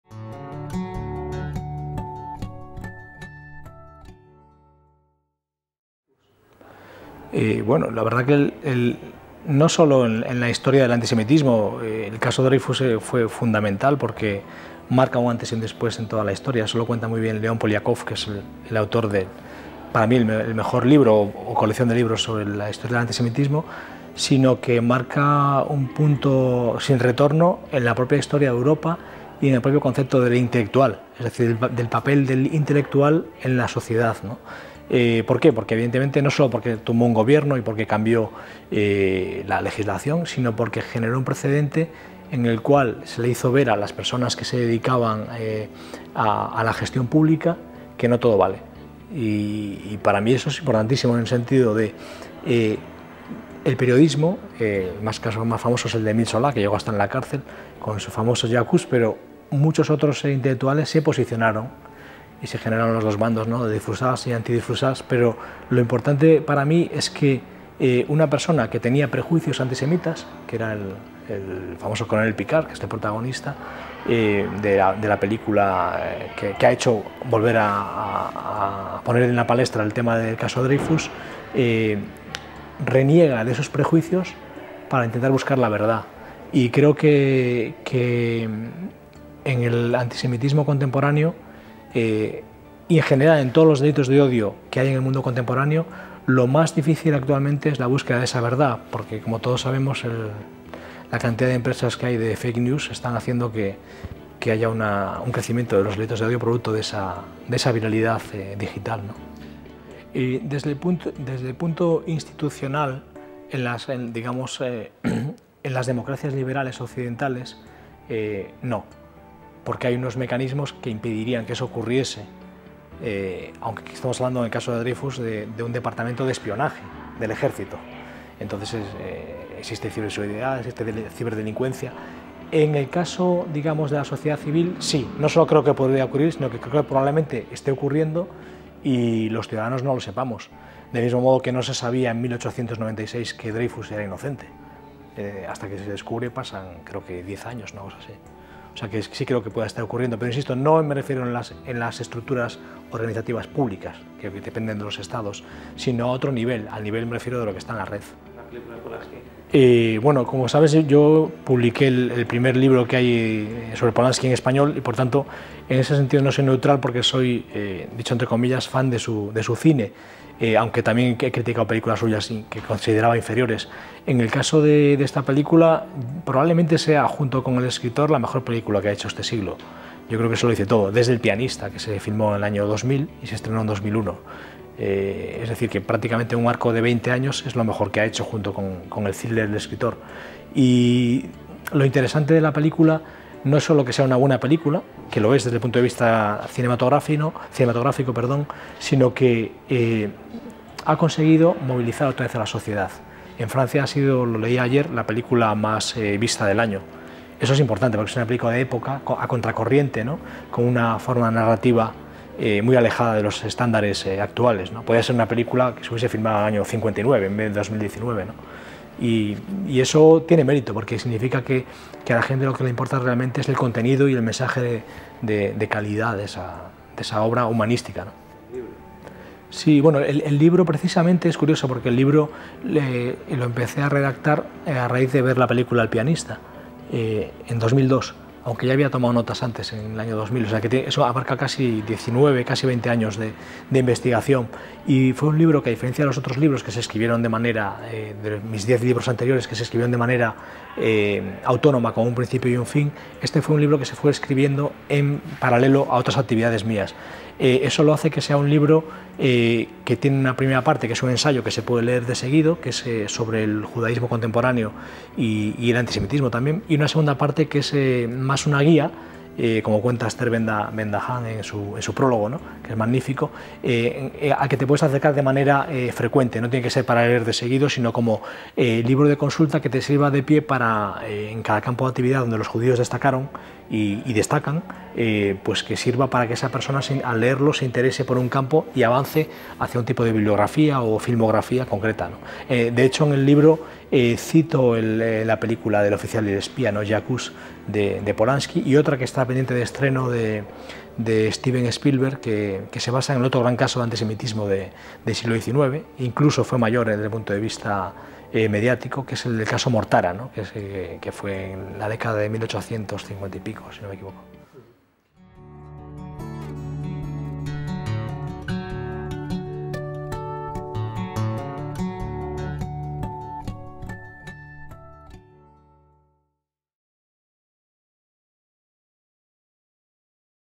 Diego Moldes nos habla sobre el Caso Dreyfus
El escritor y director de Relaciones Institucionales de la Universidad Antonio de Nebrija nos habla sobre el caso Dreyfus, sobre la película que realizado Roman Polanski sobre el mismo, y sobre su último libro "Cuando Einstein encontró a Kafka".